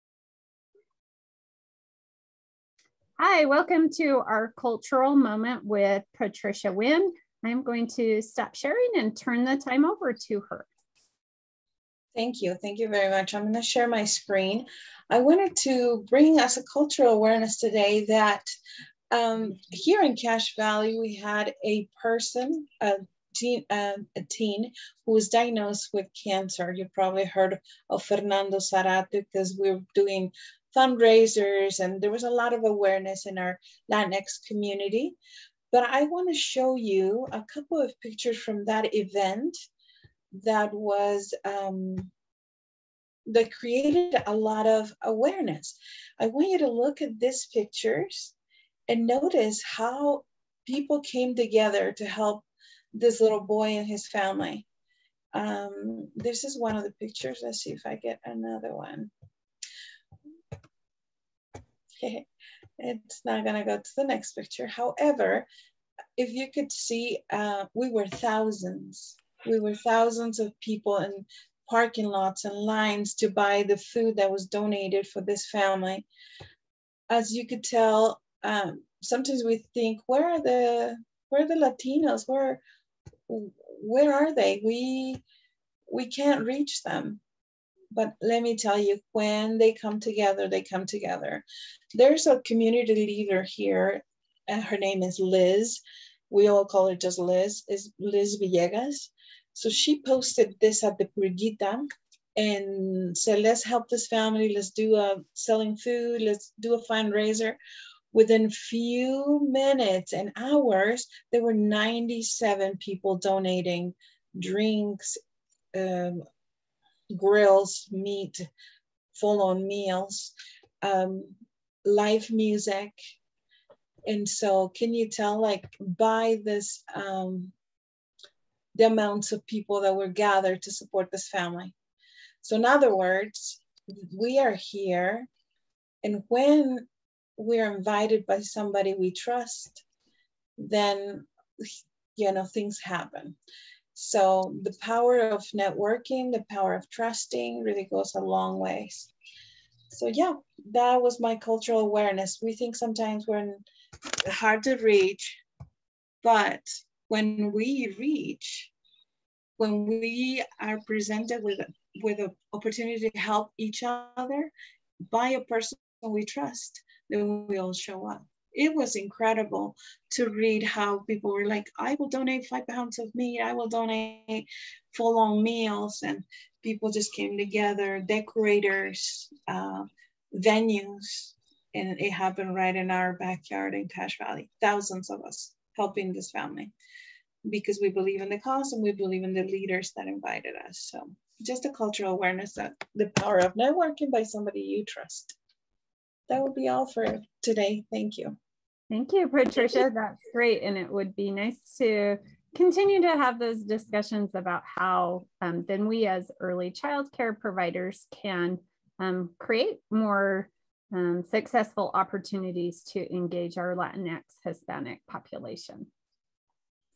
cultural moment 3 4 22
Patricia Winn shares how the Lantinx/Hispanic population joins together to help out members of their community.